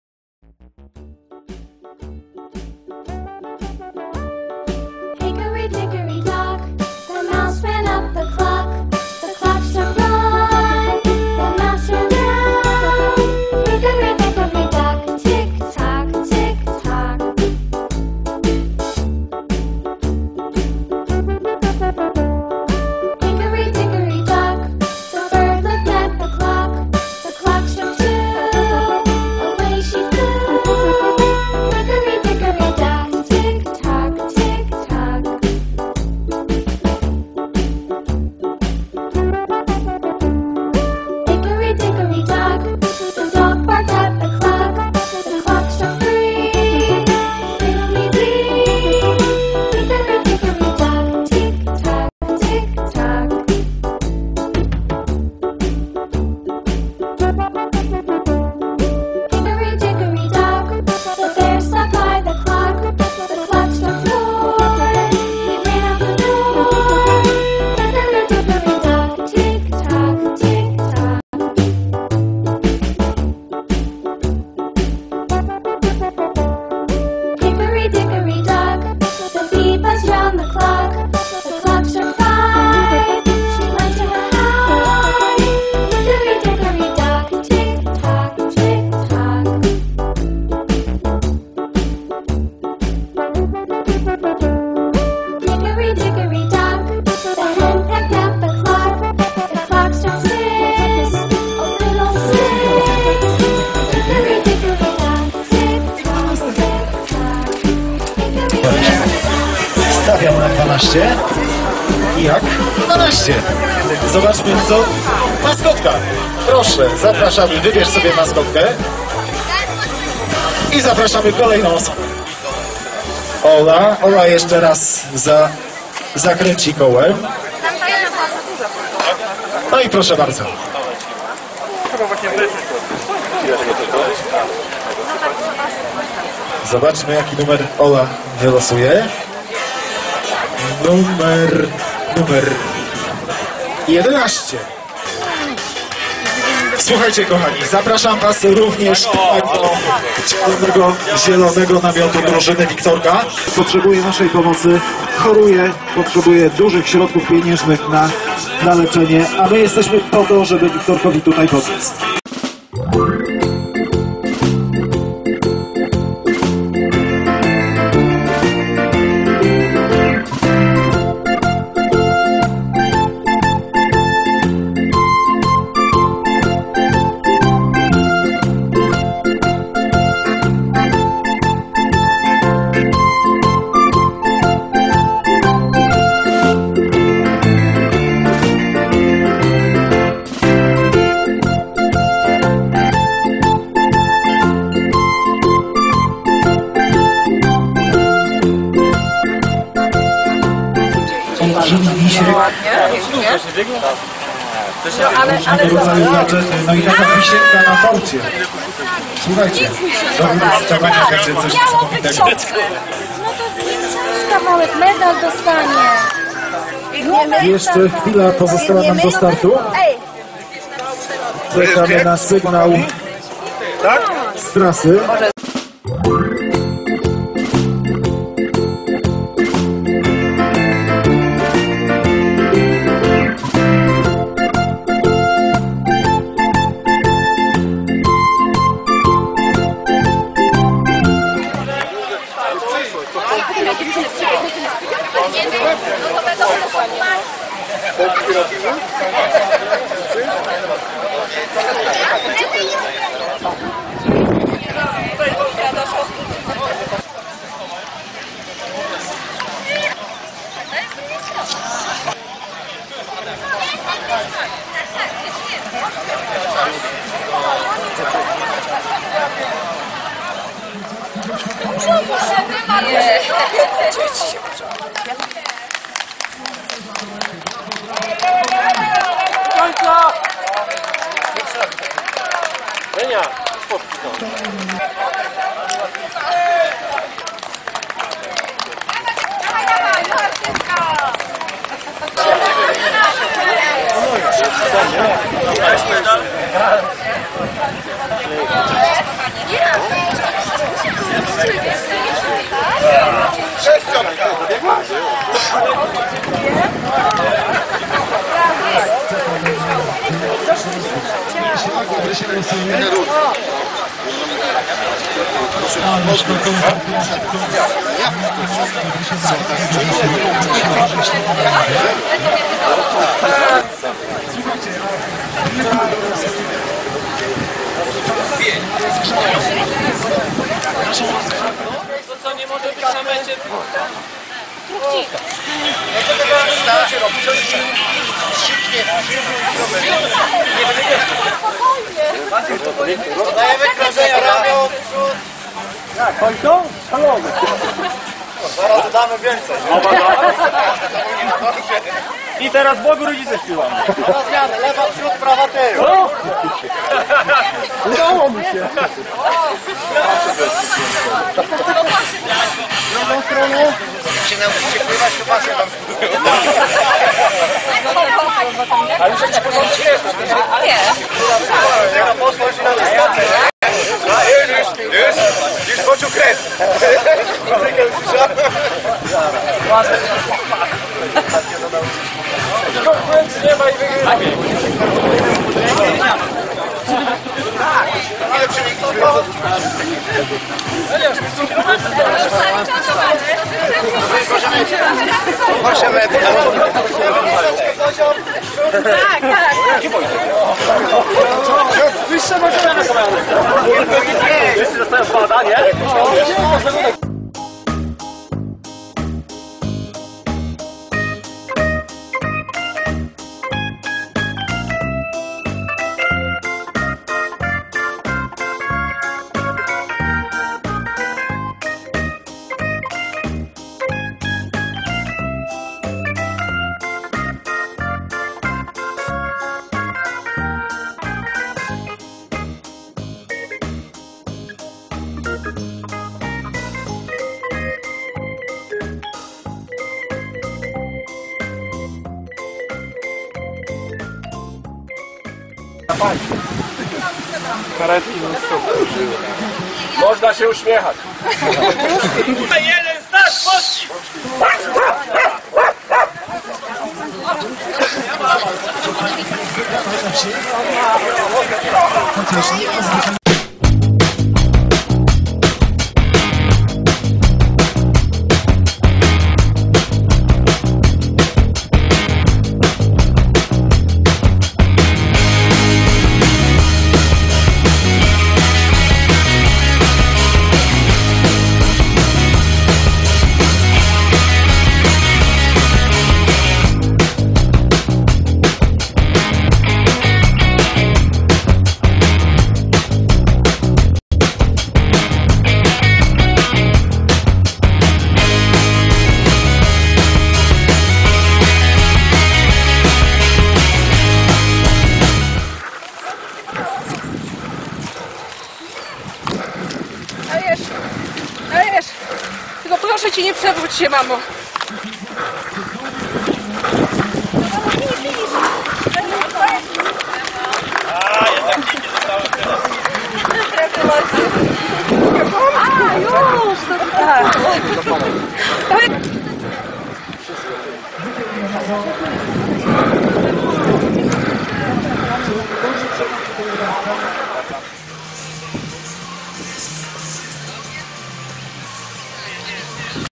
JAWOR - Biegam - Pomagam dla Wiktorka
Biegi charytatywne - moim zdaniem – są wyższą formą rekreacji. W czasie biegu każdy ma świadomość, że bezinteresownie pomaga drugiemu człowiekowi. Tak było 7 lipca 2019 roku w Jaworze dolnośląskim: w trakcie rodzinnego festynu m.in. biegano i zbierano fundusze na pomoc dla ciężko chorego chłopczyka – Wiktorka. Ponieważ dystans wynosił tylko 3 kilometry, postanowiłem nie tylko filmować wydarzenie, ale także pobiec z całą grupą, trzymając w rękach kamerę i aparat fotograficzny. Zająłem wyróżniające ostatnie miejsce, wyprzedziła mnie nie tylko córka, ale także żona z kijkami. Pocieszające było to, że to Wiktorek wygrał bieg, dublując przy okazji wszystkich zawodników na tak krótkim dystansie. Tak trzymaj, chłopie, a zdublujesz kiedyś maratończyków.